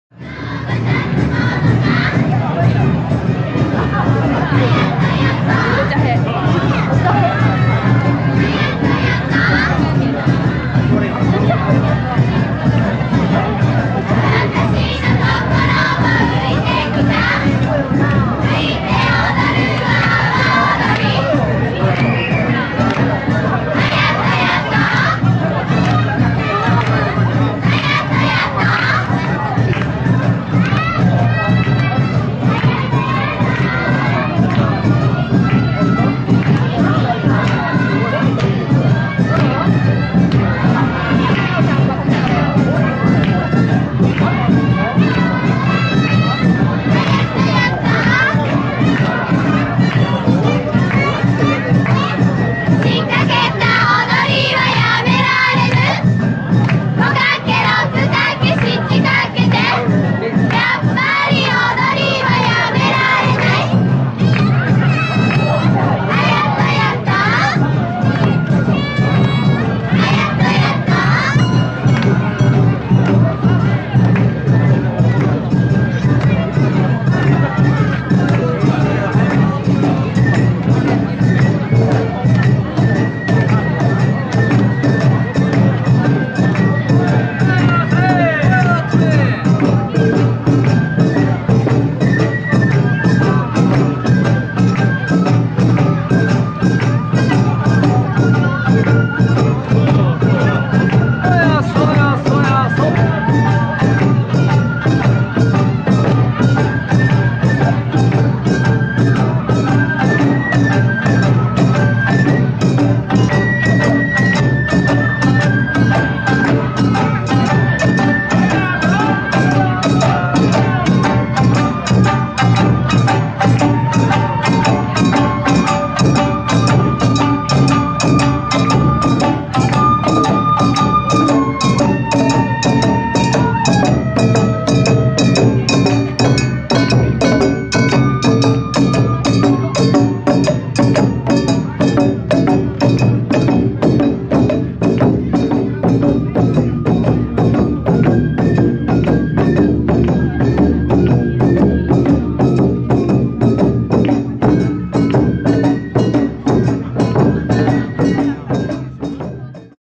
「娯茶平」流し踊り 8月16日 貞光夏まつり阿波踊り大会2014②
～つるぎ町合併１０周年記念　貞光夏祭り　阿波踊り大会～
撮影日：2014年8月16日
撮影場所：徳島県美馬郡つるぎ町貞光中央商店街
出演連：娯茶平